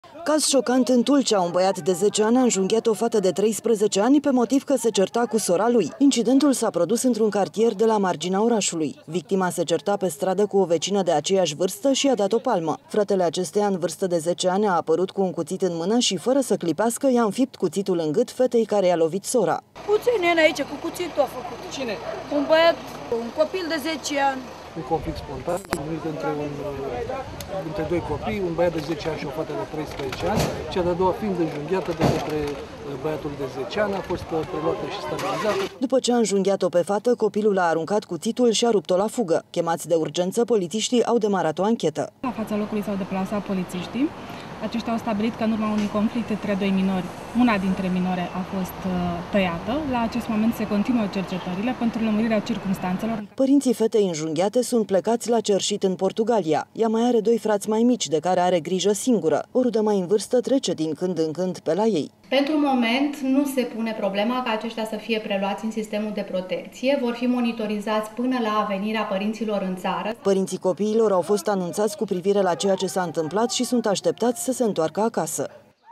Un copil de 10 ani a înjunghiat o fată de 13 ani, pe motiv că se certa cu sora lui
Caz șocant la Tulcea! Un băiat de 10 ani a înjunghiat o fată de 13 ani, pe motiv că se certa cu sora lui. Incidentul s-a produs într-un cartier de la marginea orașului. Victima se certa pe stradă cu o vecină de aceeași vârstă și i-a dat o palmă. Fratele acesteia, în vârstă de zece ani, a apărut cu un cuțit în mână și, fără să clipească, i-a înfipt cuțitul în...